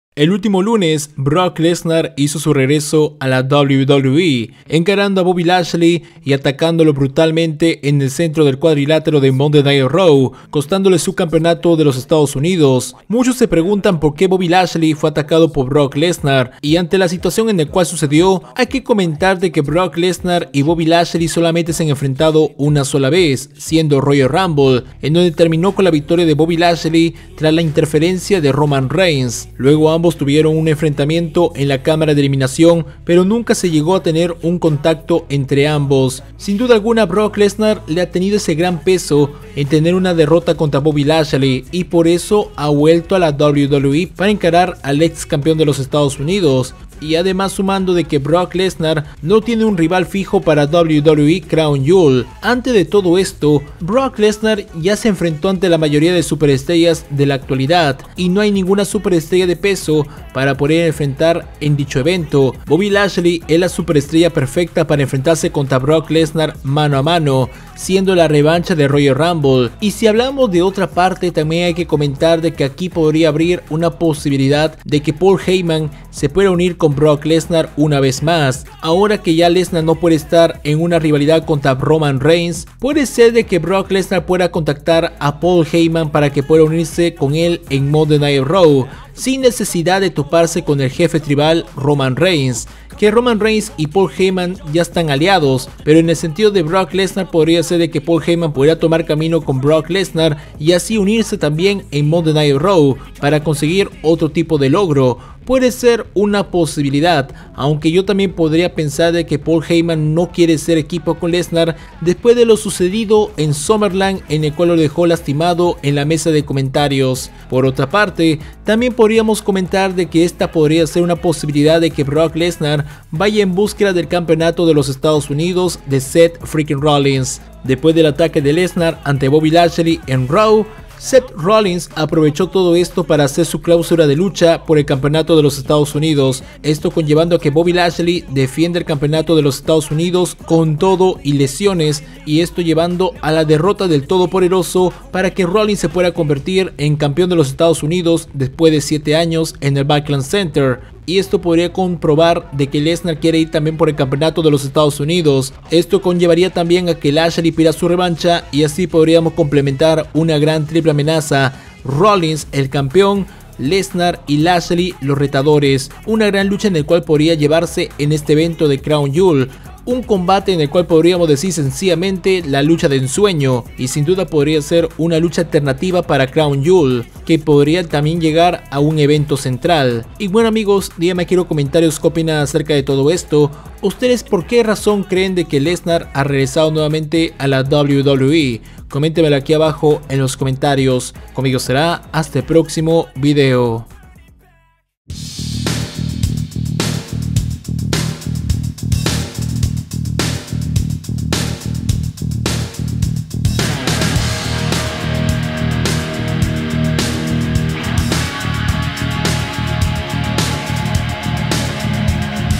PORQUE BROCK LESNAR DEBE GANAR EL CAMPEONATO DE LOS ESTADOS UNIDOS DE LA WWE ANTE BOBBY LASHLEY
El ultimo RAW, Brock Lesnar ha regresado a la WWE para atacar a Bobby Lashley y costándole el Campeonato de los Estados Unidos, para que Seth Rollins se coronara después de 7 años. Brock Lesnar confirmará su presencia para WWE Crown Jewel 2022 en Arabia Saudi contra “The All Mighty”. Esto podría cambiar, si la empresa decide realizar una amenaza triple entre Seth Rollins vs Brock Lesnar vs Bobby Lashley por el United States Champion.

WWE Crown Jewel 2022 Brock Lesnar vs Bobby Lashley